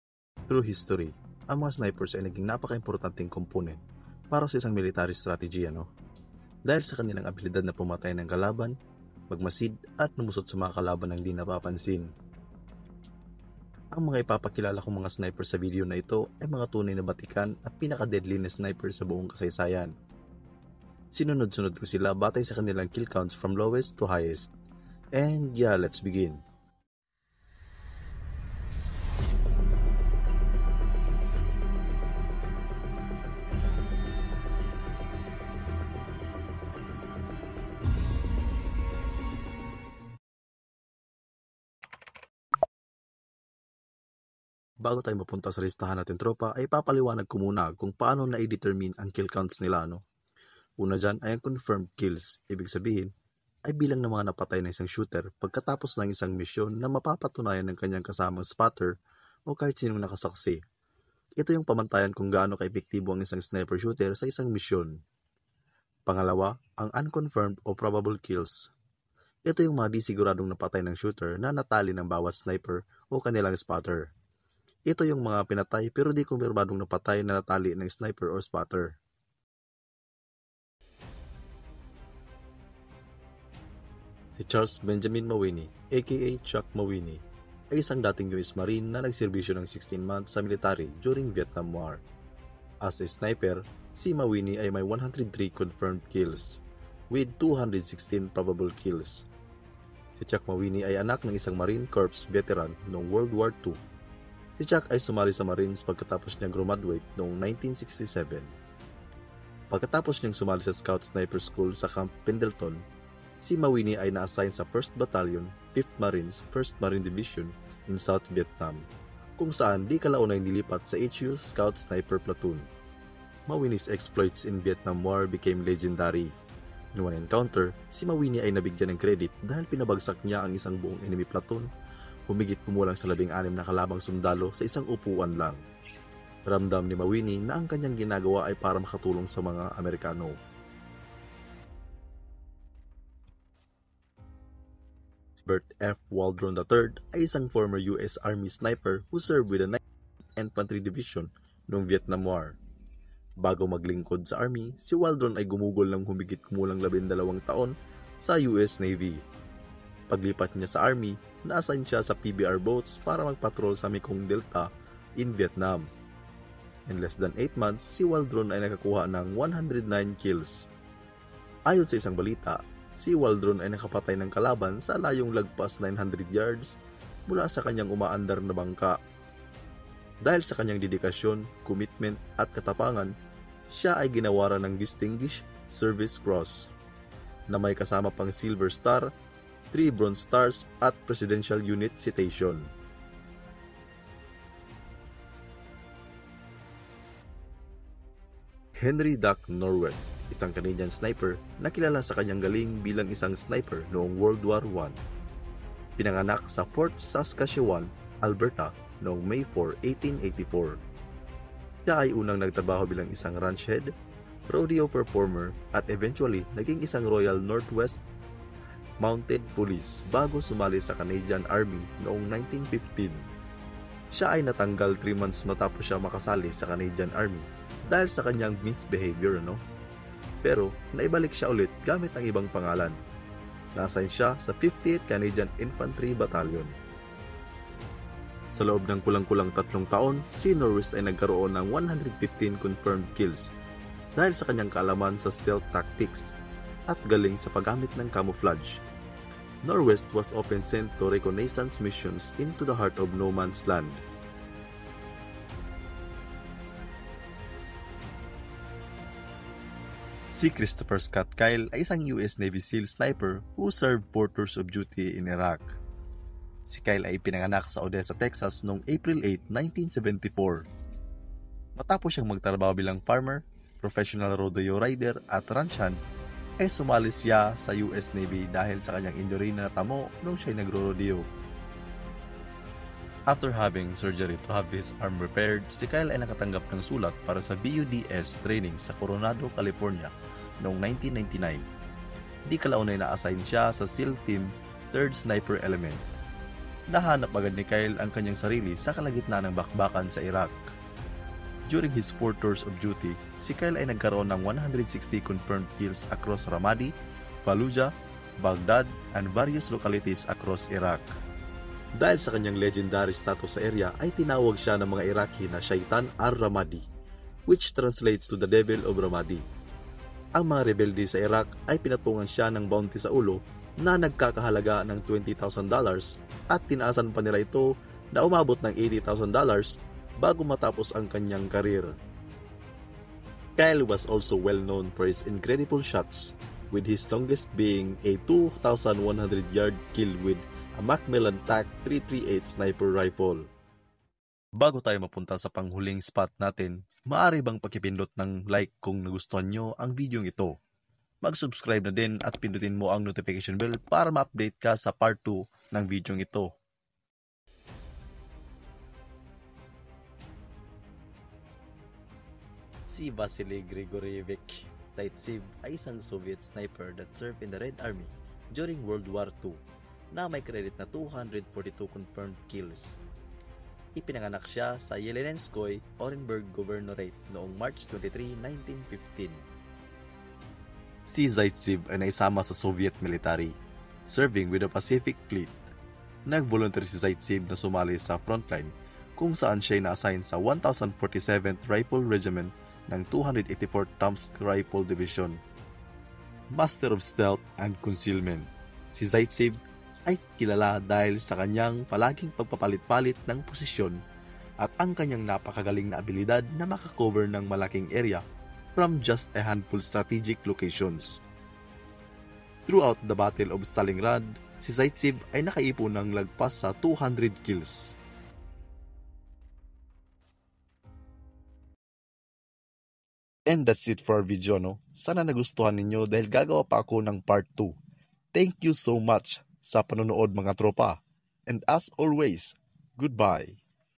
MGA PINAKAMAGALING NA SNIPERS SA KASAYSAYAN | Part 1| Vale TV
DEADLIEST SNIPERS IN HISTORY | KILL COUNTS | VALE TV

Pamantayan sa pag determine ng Kill Counts
1. Confirmed kills-- ibig sabihin ay bilang ng mga napatay ng isang shooter pagkatapos ng mission na mapapatuyan ng kanyang kasamang spotter o kahit sinong naka saksi,,,,, ito yung pamantayan kung paano ka epektibo ang isang sniper shooter sa isang mission.
2. Unconfirmed or Probable kills-- ito yung mga di seguradong napatay ng shooter na natallie ng bawat sniper. Ito ay yung mga pinatay pero di kompermadong napatay talaga ng sniper.

Part 1 List of Snipers

1. Charles Benjamin Mawhinney - 103 CK
2. Adelbert F. Waldron III - 109
3. Henry "Duck" Norwest - 115
4. Christopher Scott Kyle - 160
5. Vasily Grigoryevich Zaytsev - 242

They are just the First batch of the best and deadly snipers in history.

I will be making part 2 of this
Ito ay para magkaroon kayo ng Kaalaman kung sino ang mga pinaka sikat at deadly na snipers sa buong kasaysayan

Part 2: DEADLIEST SNIPERS IN HISTORY | KILL COUNTS | VALE TV

Deadly Snipers in History.